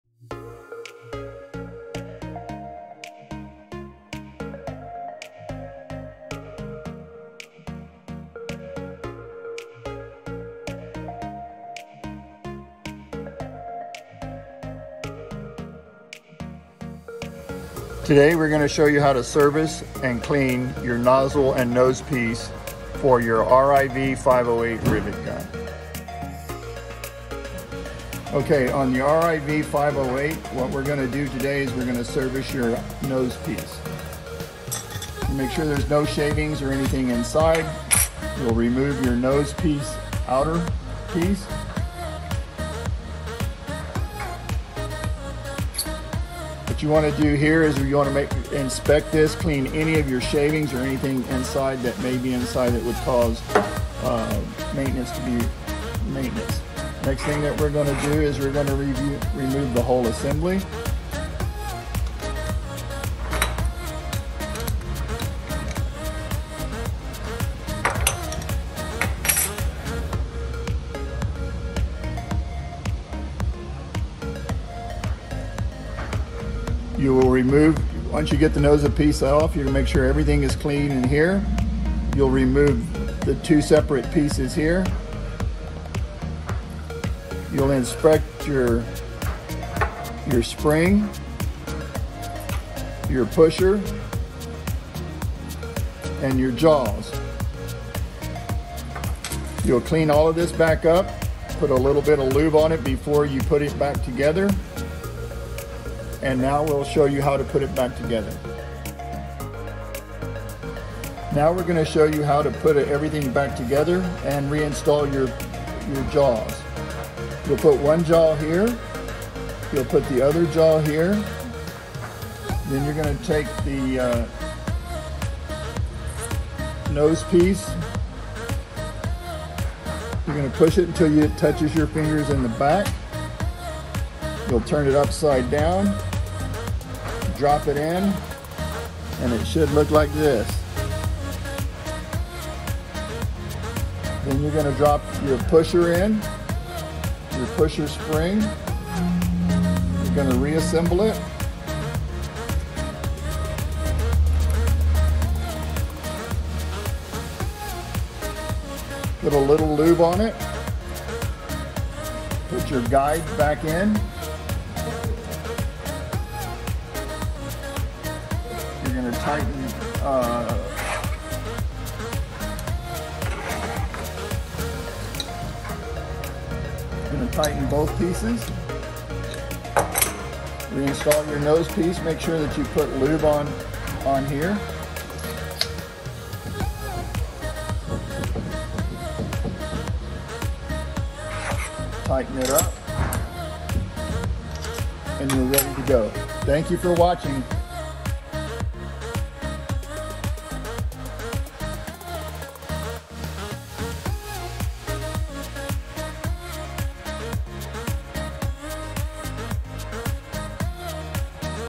RIV508 Blind Rivet Gun Jaw service Instructional video
RIV508 Blind Rivet Gun Jaw service Instructional video, all blind rivet guns should have a regular service schedule to remove metal shavings and dust. The failure to do this could lead to nose piece damages and total gun failure. Grade A Tools wants to insure that your rivet gun has a good long life and gives you years of service.